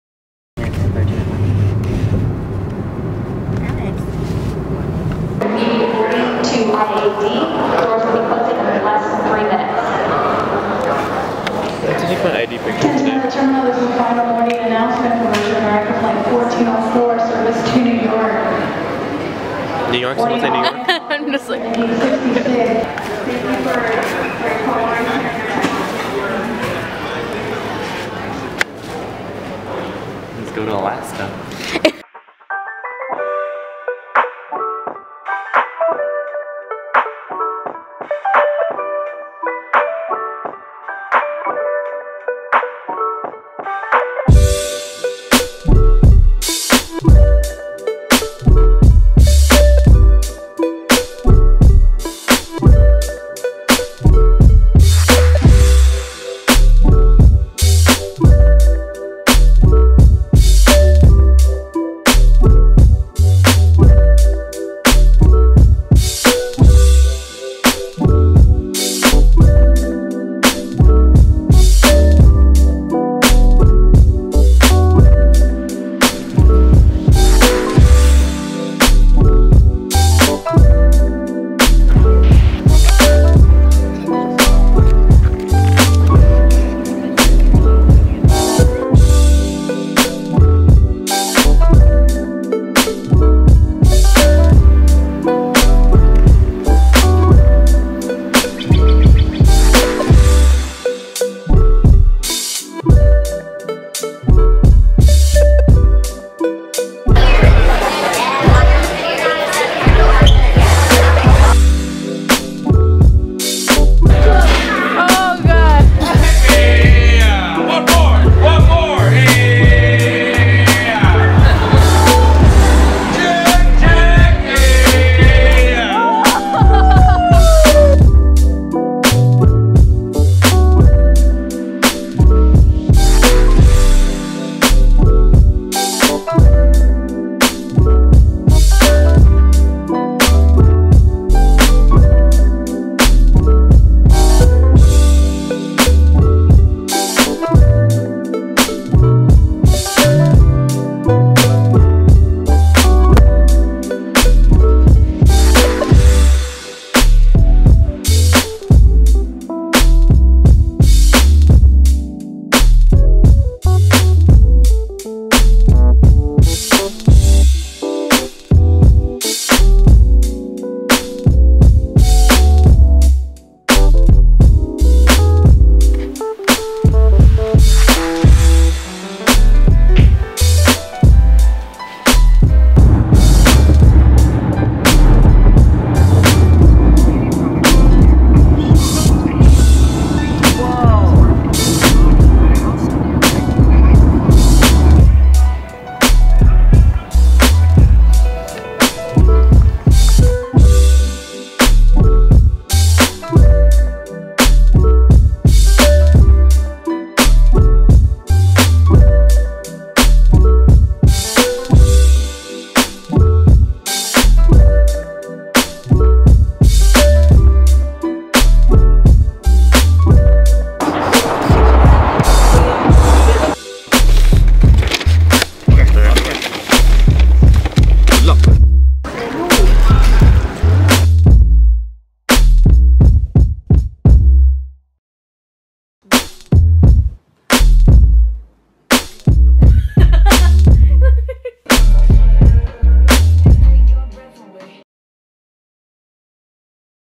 LA x SEA Vlog
I'm two months late. BUT its okay! we are back and ready for action!!! Here is my Seattle Vlo, I say goodbye to my best friend Alex attending Seattle University! Just a quick recap basically. Hope you enjoy! NEXT VLOGMASSSS